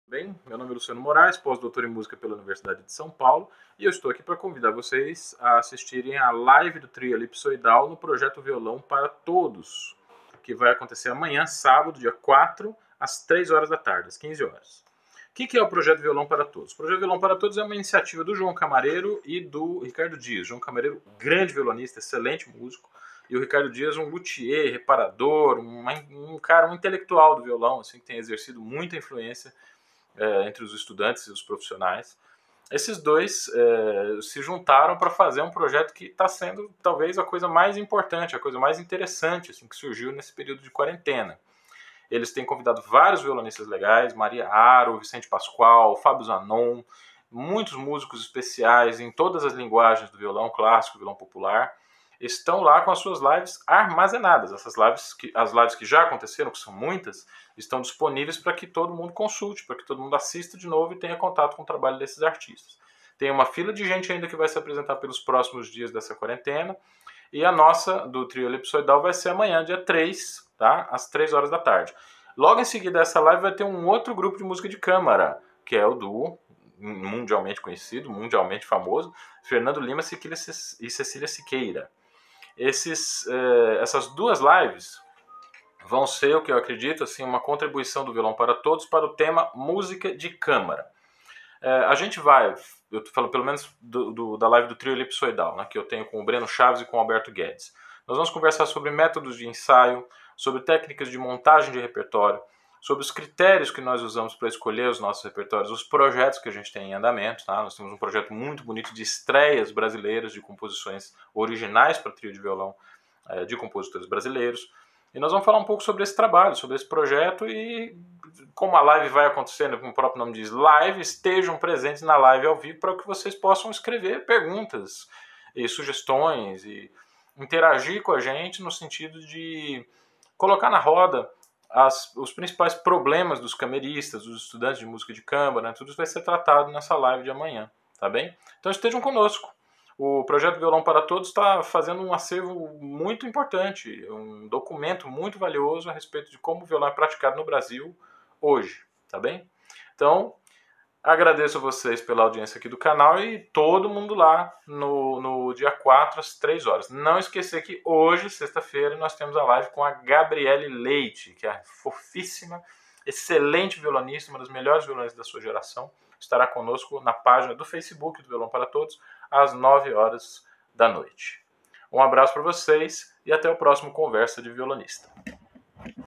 Projeto Violão Para Todos, no Facebook. Trio Elipsoidal dia 4 às 15, Gabriele Leite HOJE, às 21!!!!!
Convite para atividades de violão na quarentena!